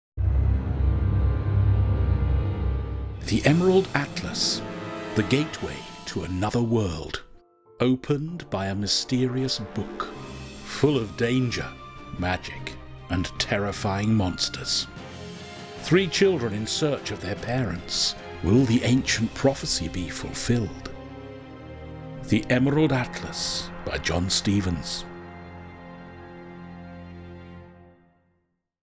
John Stephens - The Emerald Atlas
"The Emerald Atlas" by John Stephens is the first book in the series of "The Books of Beginning". Ten years on, Kate, Michael and Emma have grown up in a string of miserable orphanages, and all memories of their parents have faded to a blur. Arriving at Cambridge Falls, the children quickly realise there is something different about this place - and Kate feels sure she has seen the dark, crooked house before.